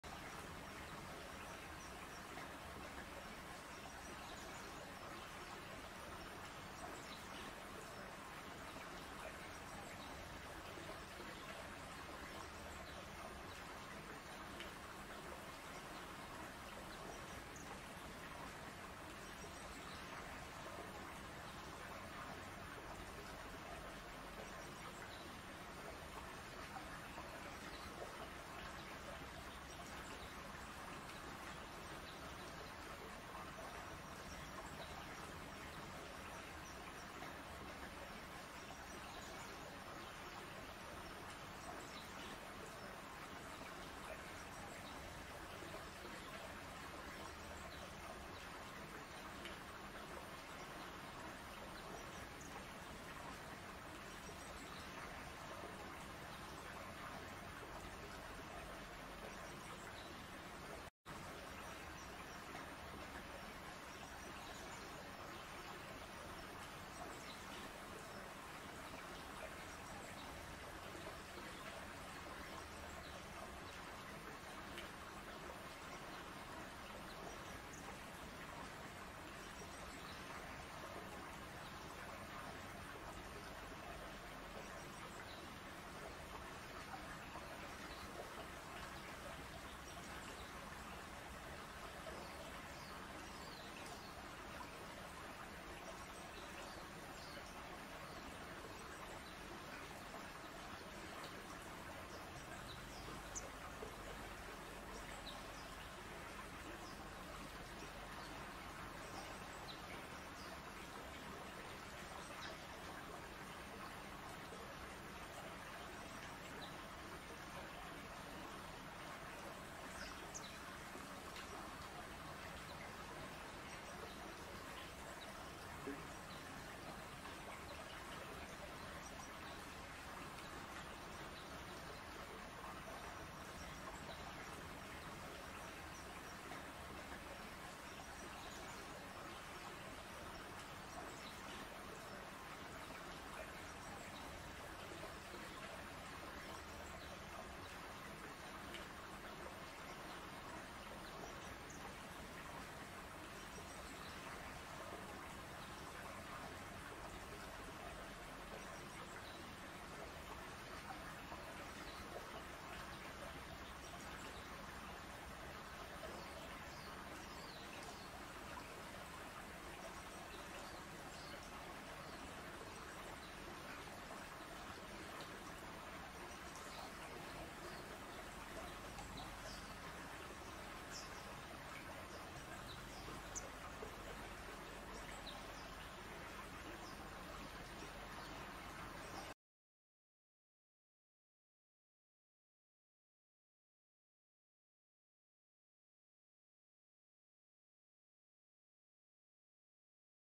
Boxing, diving, and swimming with Mom!
Hope you enjoy this little clip of the Toledo Polar Bear Cubs!

🧸️ These two bundles of joy, were born November 2022 to 24-year-old mother Crystal and 18-year-old father Nuka at the Toledo Zoo in Ohio.   Their names are Kallu and Kallik - Inuit words for Lightning and Thunder, which the zoo said pretty much explains their characteristics.

A huge thank you to the Toledo Zoo for opening up their facility so that the entire world can follow along their journey.  Thank you!  ❤️

Source of clip:  Toledo Zoo Polar Bear Cam